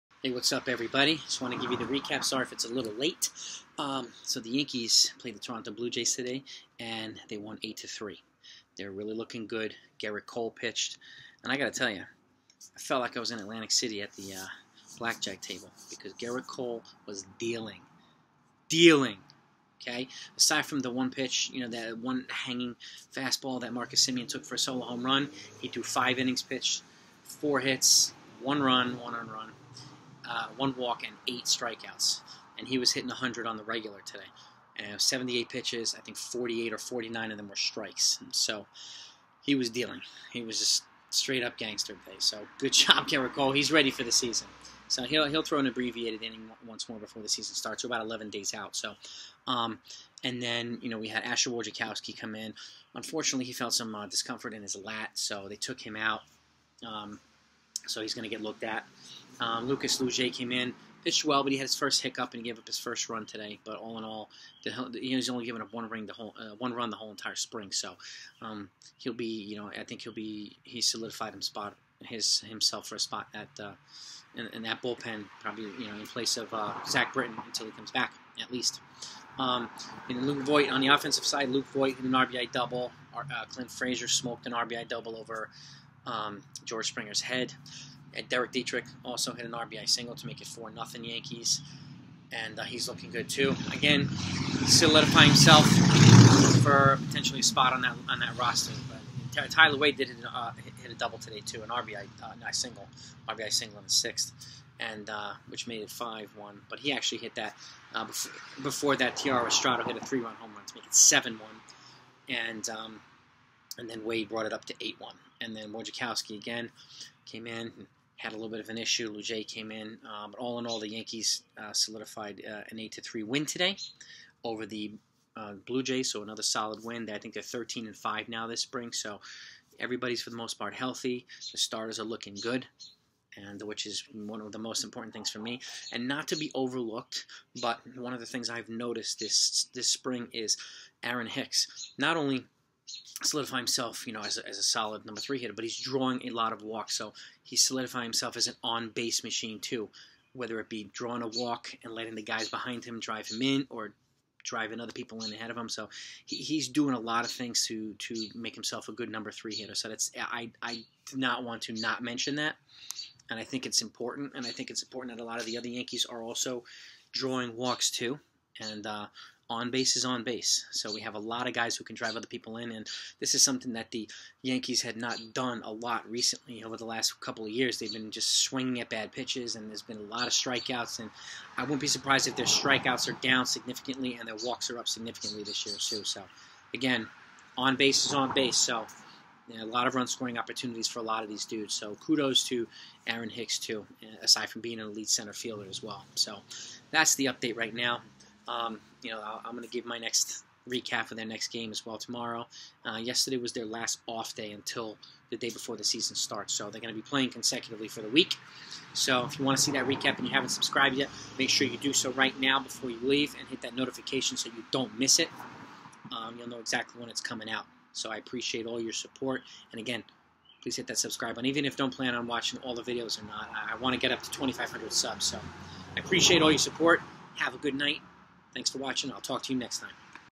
Yankees 8 - Blue Jays 3: Post-Game Recap || Gerrit Cole is READY TO GO! || New OBP. Philosophy?
Yankees 8 - Blue Jays 3 Post-Game Recap